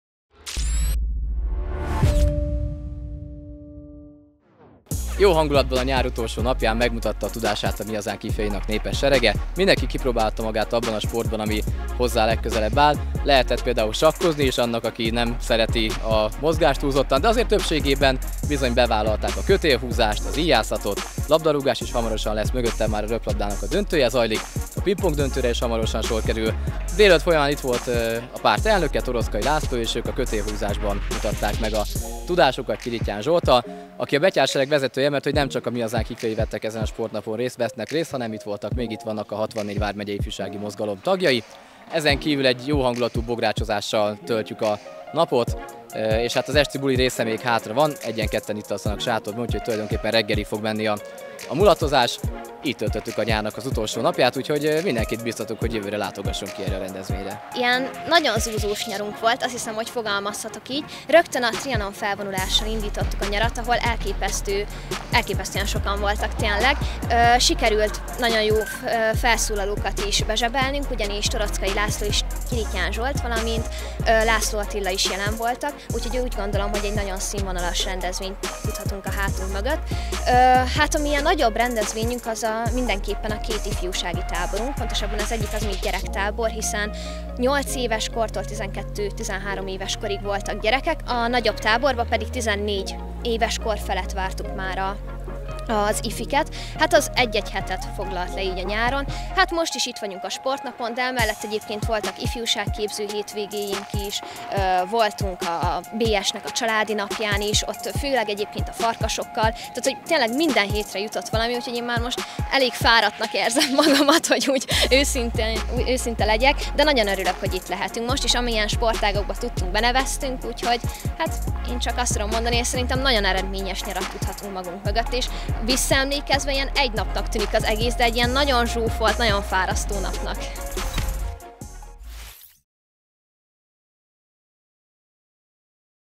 Ifjak Sportnap, egy mozgalmas nyár lezárása
Idén is megrendezésre került az Ifjak Sportnap, ahol többféle sportágban is kipróbálhatták magukat a résztvevő ifjúsági szervezetek tagjai.